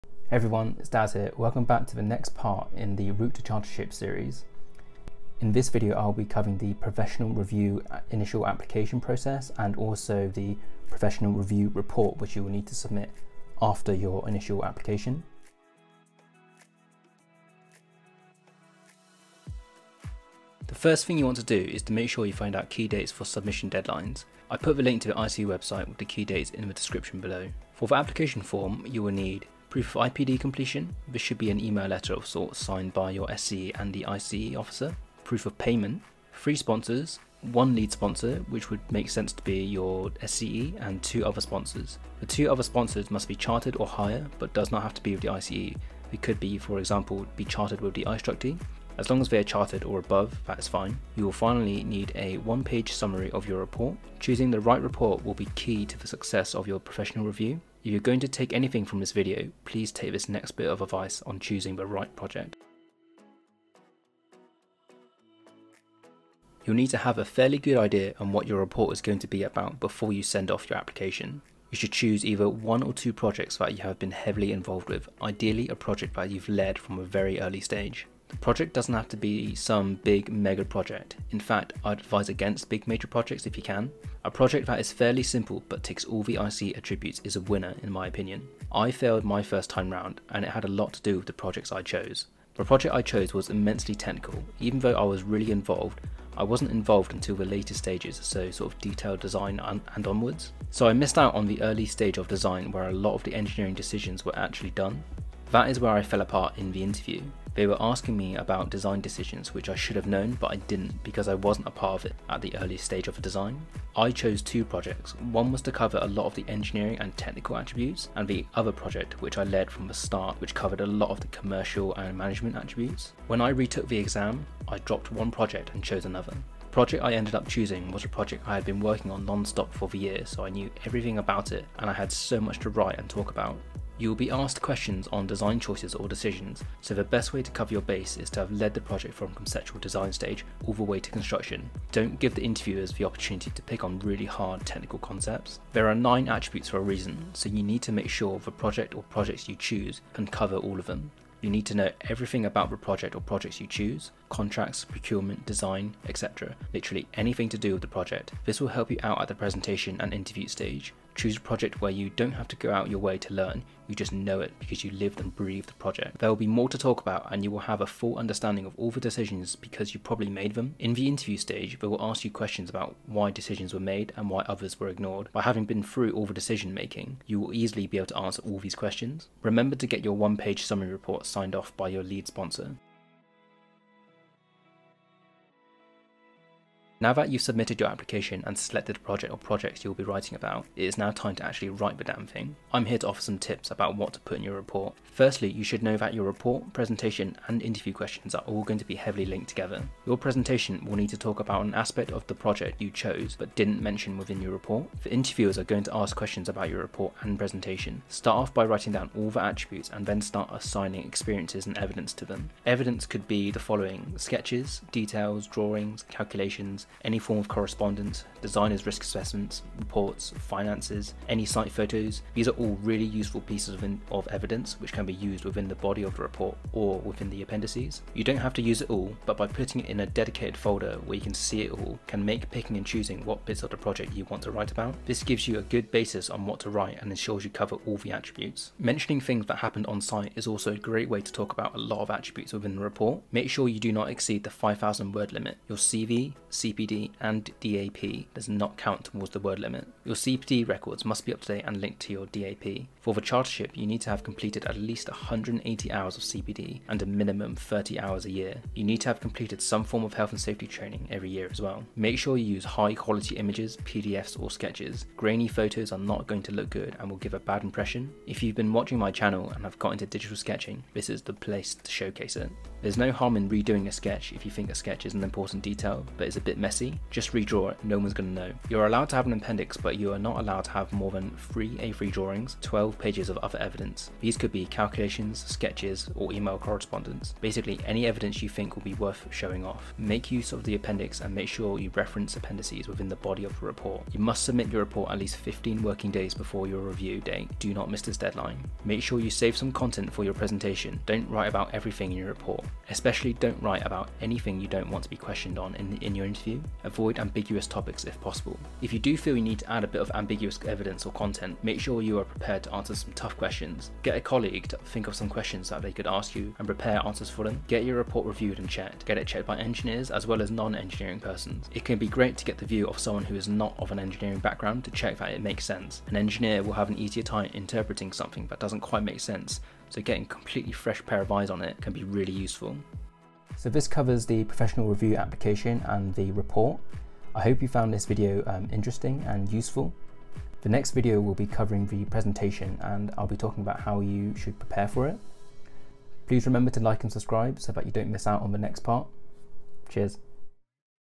How to become a qualified engineer with the ICE - Part 2 - Application & Report
Welcome back to part 2 in the "How to become a qualified engineer with the ICE".

I go through the application process once you have your IPD signed off and then provide some insight on what to write in your report.

0:00 Intro
0:20 CPR Application
1:02 Choosing your project
3:03 Writing your report

Gear I Use:
iPad Pro
Magic Keyboard for iPad
Airpods Pro
Tonor Mic
Lav Mic
Go Pro Hero 7
San Disk SD Card